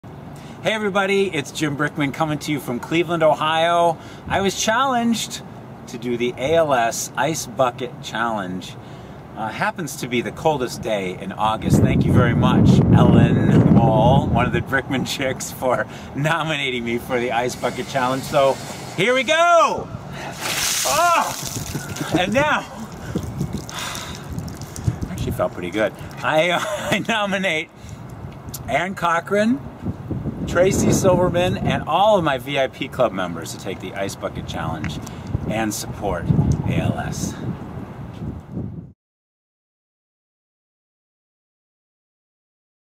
Jim Brickman - ALS Ice Bucket Challenge
Jim Brickman was challenged to the Ice Bucket Challenge to support ALS!